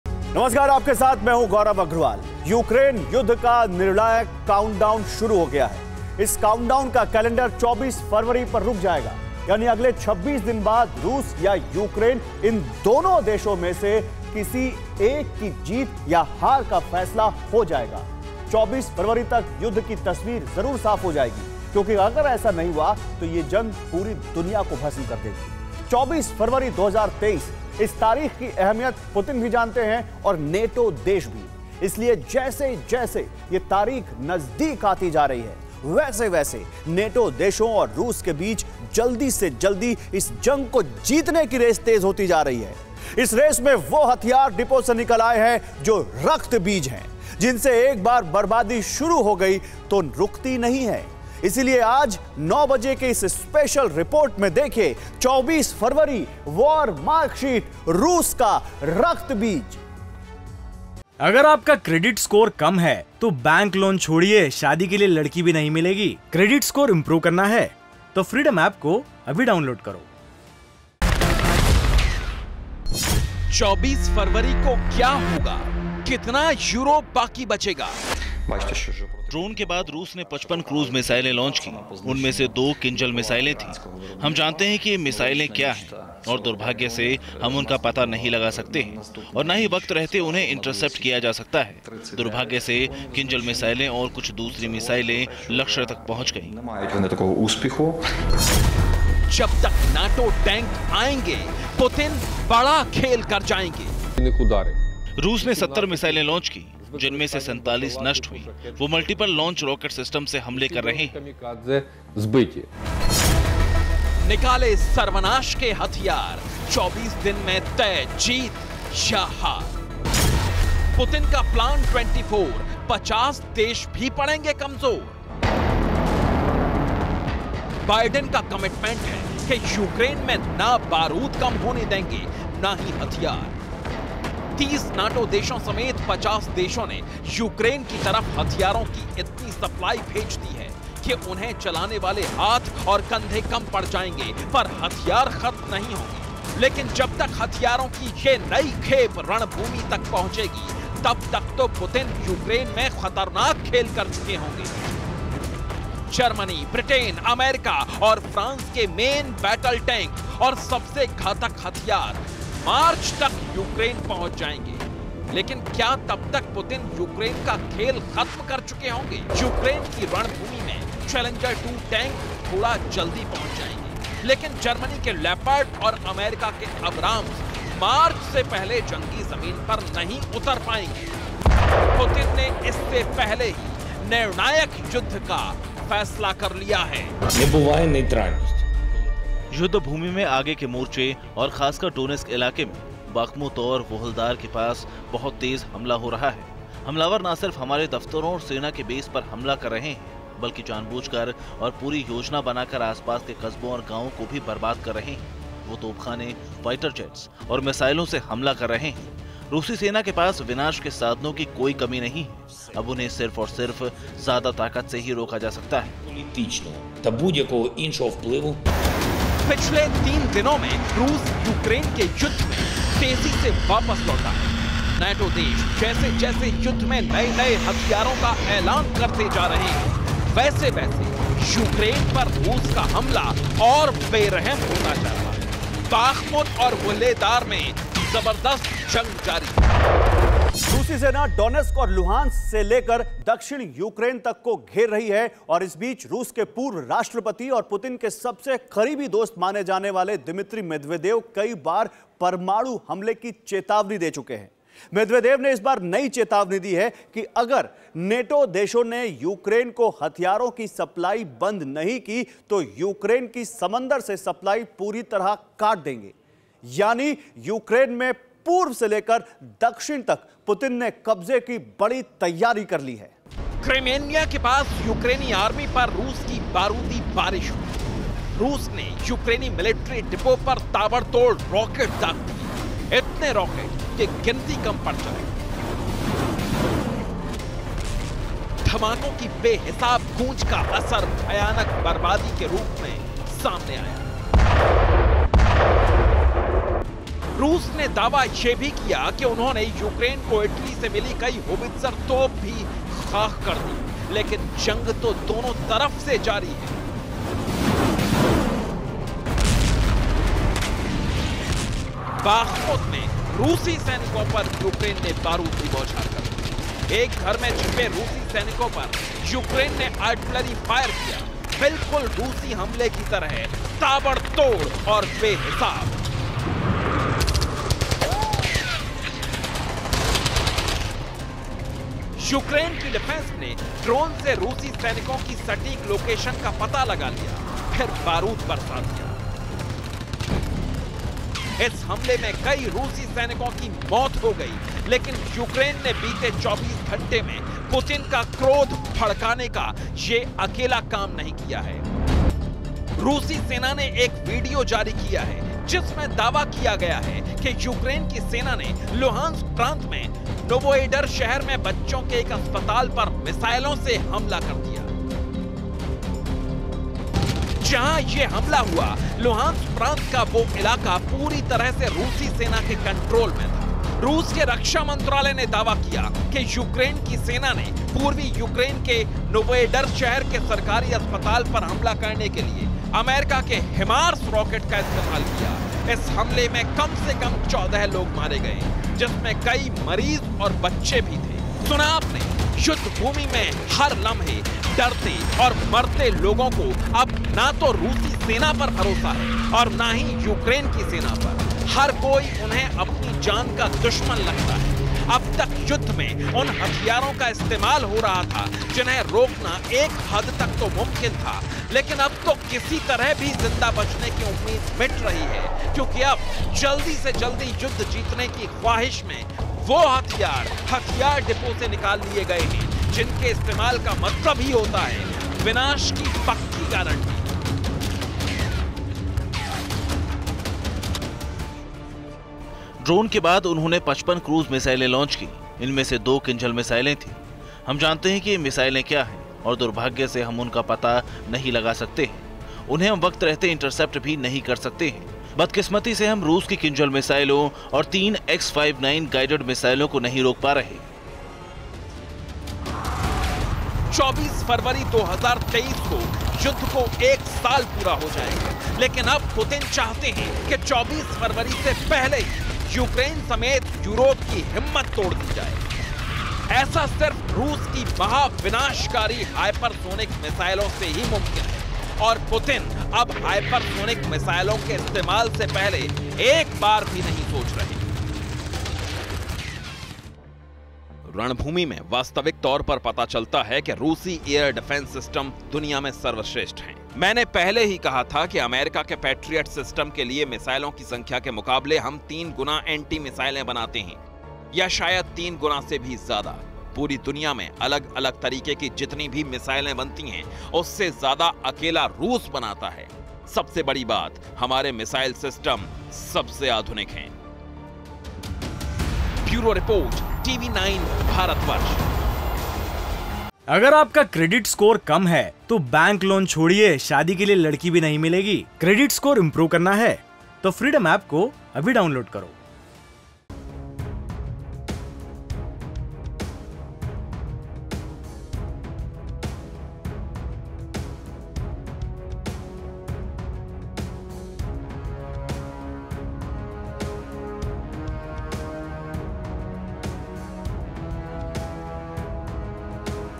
Russia-Ukraine War : NATO और रूस के बीच जल्दी से जल्दी जंग जीतने की रेस तेज | Putin | Zelenskyy
यूक्रेन युद्ध का निर्णायक काउंटडाउन शुरू हो गया है... इस काउंडाउन का कलेंडर 24 फरवरी पर रुक जाएगा.. यानी अगले 26 दिन बाद रूस या यूक्रेन, इन दोनों देशों में से किसी की जीत या हार का निर्णय हो जाएगा.